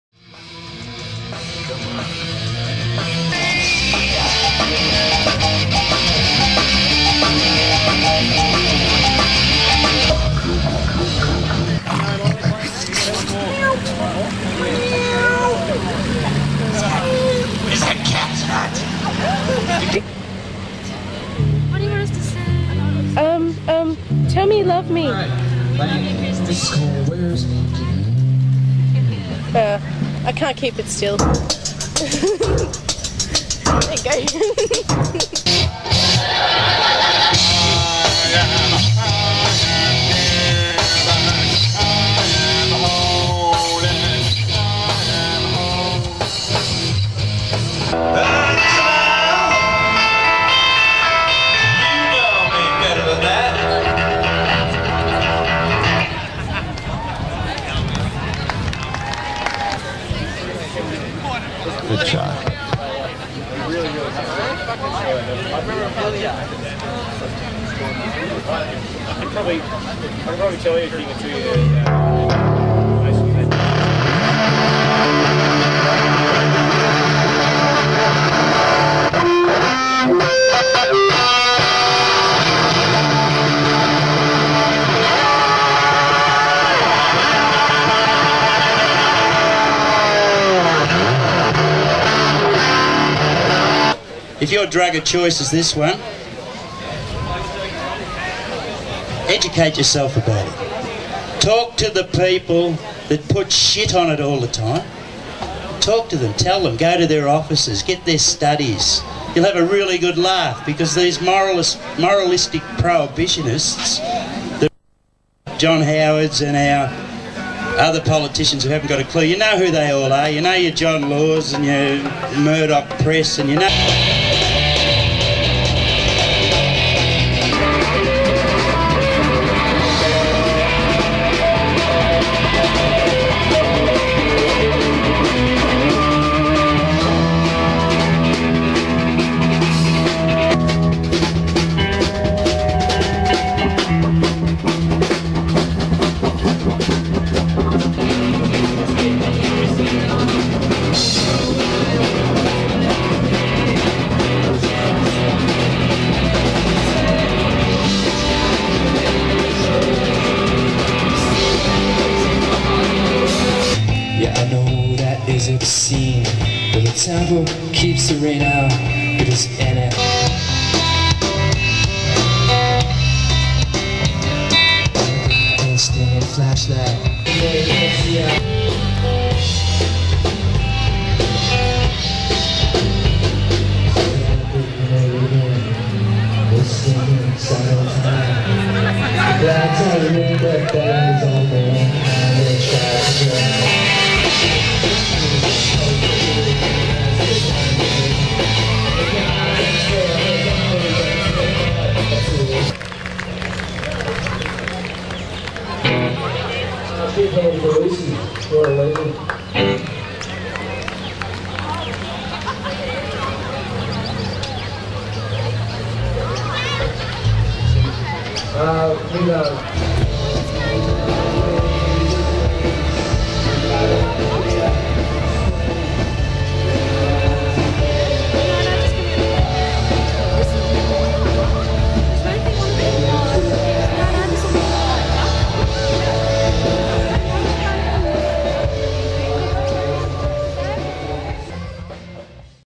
Hemp Festival 1997
A music day in King Edward Park to finish the festival. Videos shows a bit of Clockwork, Kim Salmon and The Wash